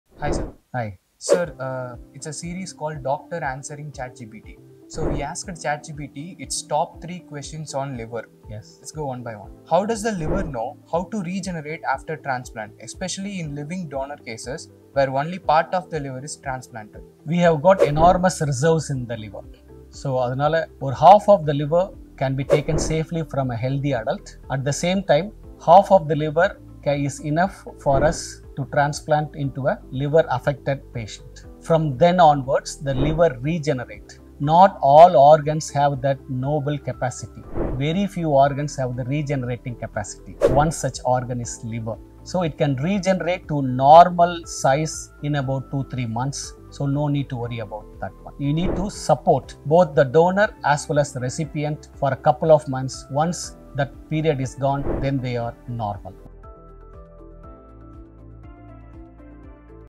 Doctor Answering ChatGPT | Ft. Dr. Senthilnathan | Part - 1
Usually, it’s us who seek answers from ChatGPT. But this time, we are turning the tables as our doctors answer questions from ChatGPT.

Presenting the first video in our new series “Doctor Answering ChatGPT.”

In this episode, Dr. Senthilnathan addresses an important liver related question posed by ChatGPT.